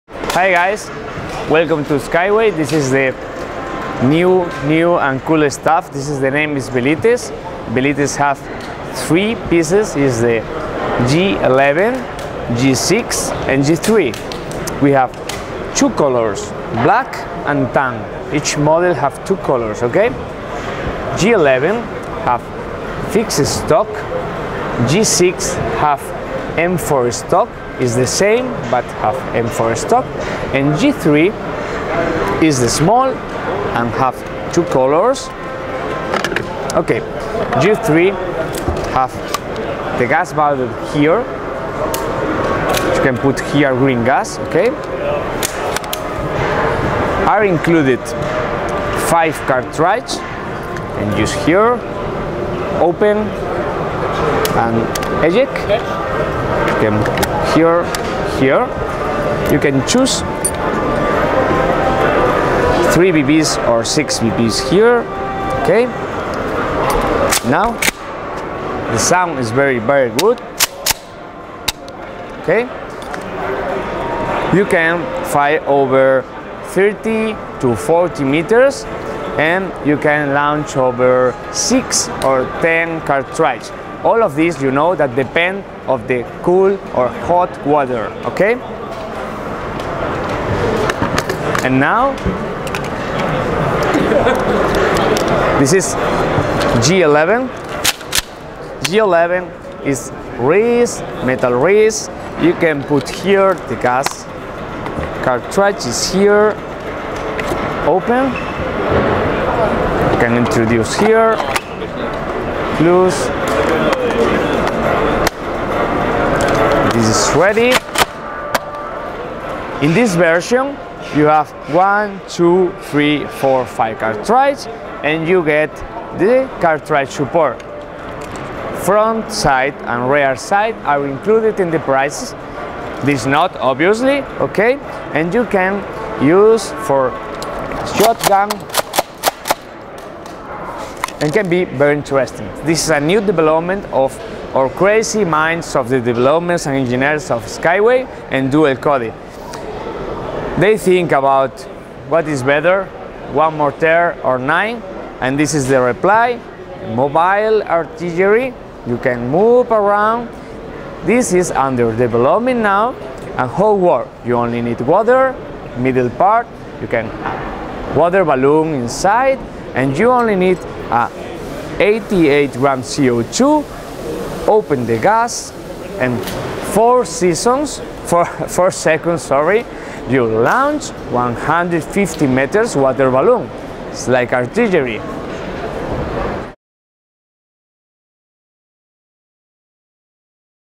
IWA 2017: SKW Airsoft Secutor Velites shotguns
At IWA 2017 the distributor and manufacturer SKW Airsoft introduced their upcoming gas-powered shotgun series Velites from the Secutor brand.

They also showed the prototype of their CO2-powered multi launcher.